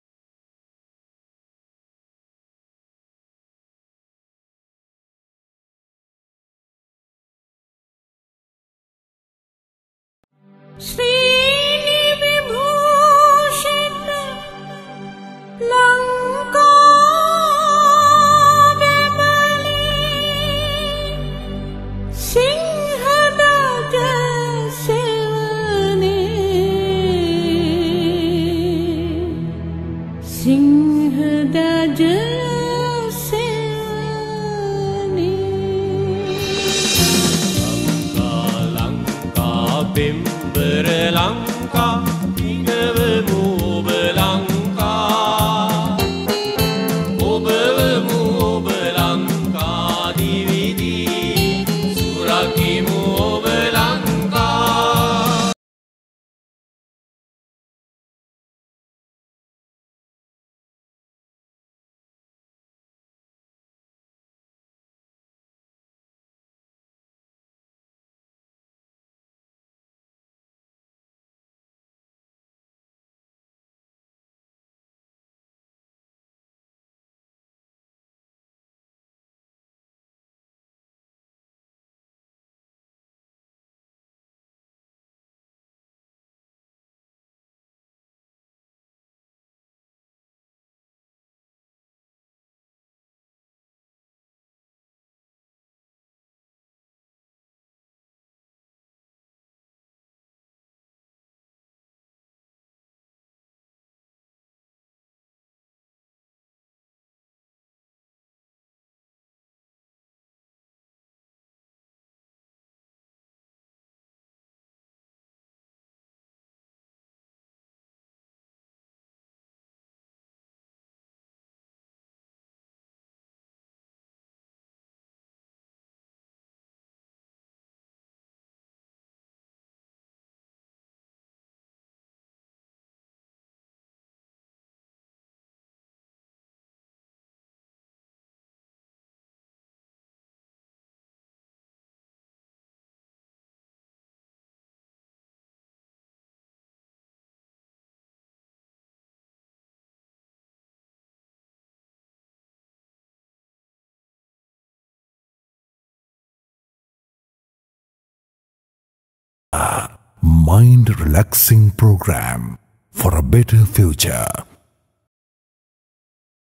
Wall Art Trend In Sri Lanka....
Gratitude to the Sri Lankan Girls & Boys who offering their full effort to make our country beautiful.